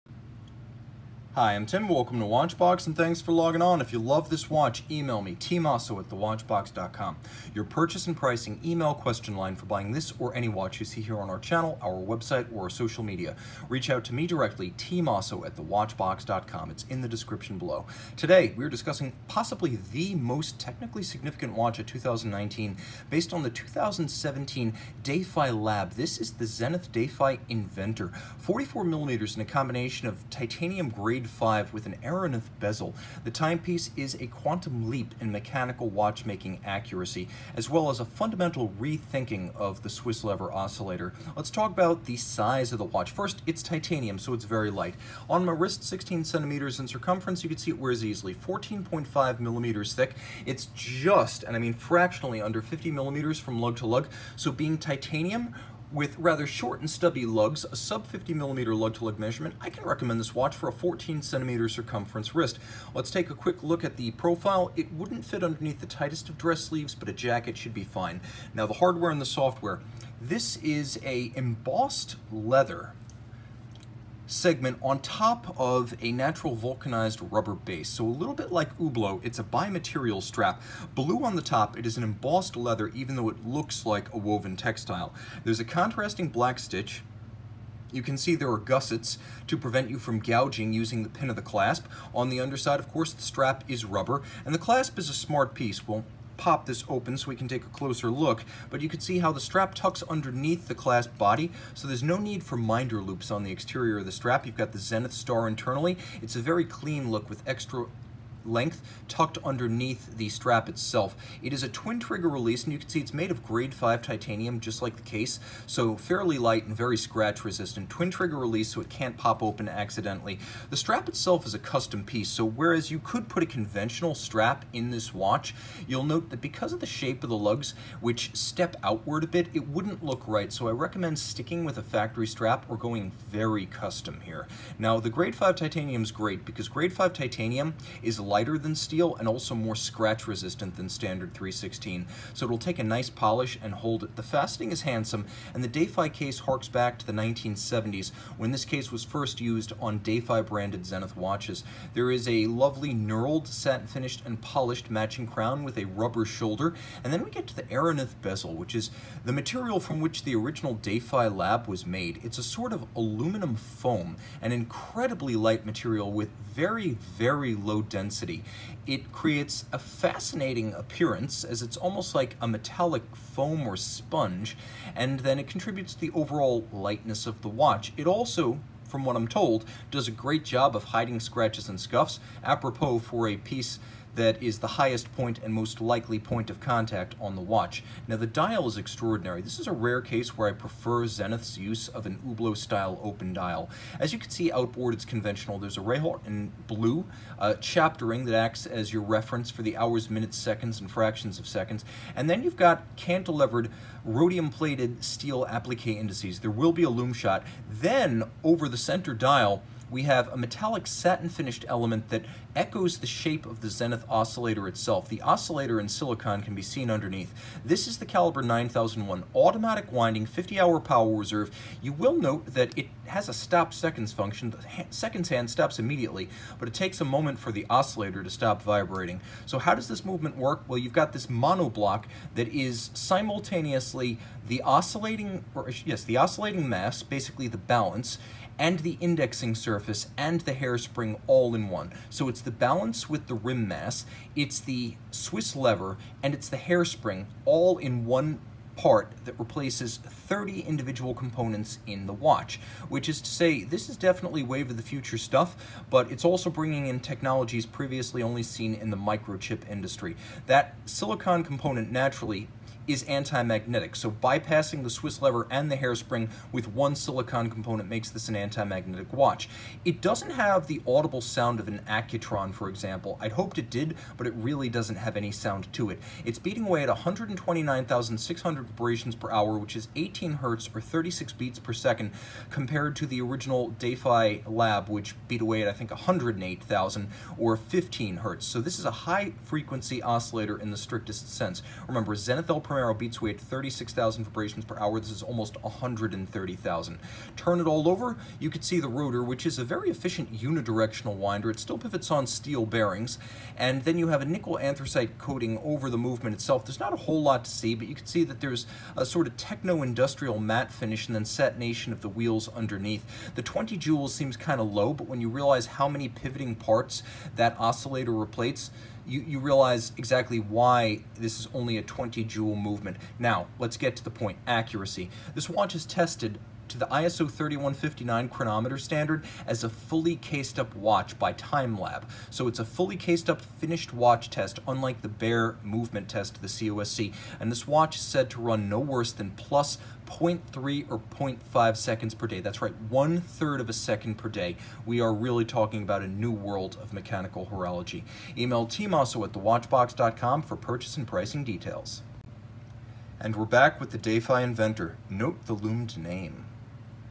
Zenith Defy Inventor 95.9001.9100/78.R920 Zenith Watch Review
The Zenith Defy Inventor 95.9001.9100/78.R920 is encased in 44mm of titanium surrounding an openworked dial on a blue fabric/black rubber strap. Features of this Zenith Defy Inventor include silicon resonator, TimeLab chronometer certification, hours, minutes and seconds.

For complete details, watch the full Zenith Defy Inventor review by Tim Mosso!